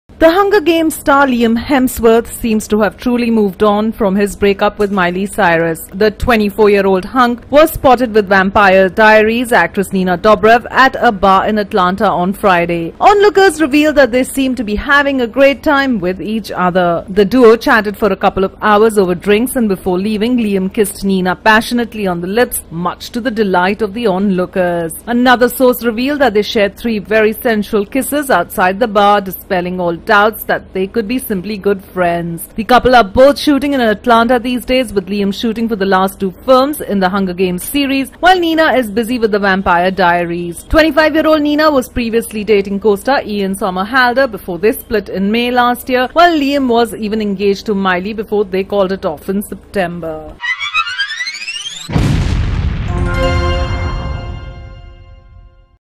Liam & Nina Share Passionate Kisses At A Bar
Liam Hemsworth and Nina Dobrev were spotted together at a bar. They exchanged passionate kisses in public, sparking off rumors that they could be a couple. Check out the video to find out what Liam and Nina were up to!

For the latest events, news and updates in Hollywood,